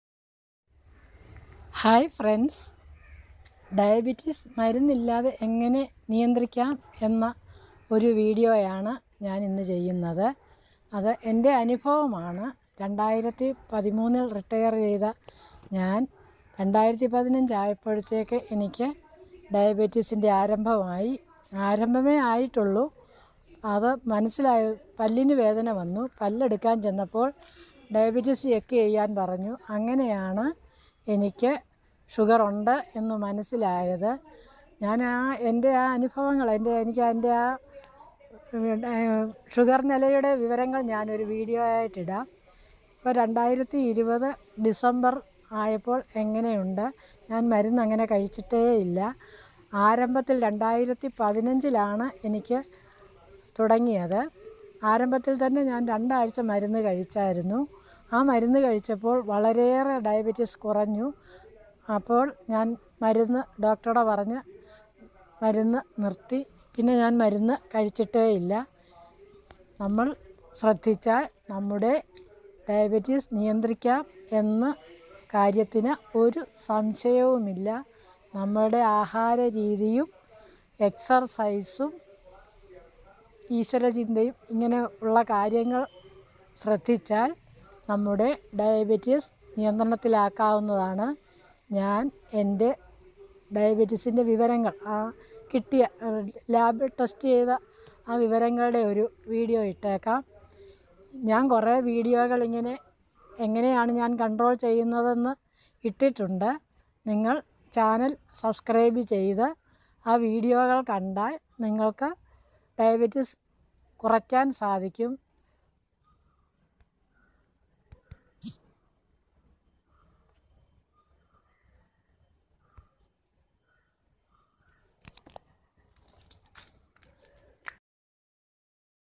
മരുന്നില്ലാതെ എങ്ങനെയാണ് diabetics നിയന്ത്രിക്കുന്നത് എന്റെ അനുഭവം
പ്രമേഹം നിയന്ത്രിക്കുന്നതിന്
1, ആഹാരത്തിൽ carbohydrate ന്റെ അളവ് കുറയ്ക്കുക
2, പതിവായി Exercise, walking ഇവ ചെയ്യുക
3, മനസ്സ് ശാന്തമാക്കി വെയ്ക്കുക. അതിനായി യോഗ, ഈശ്വരാരാധന ഇവ ചെയ്യുക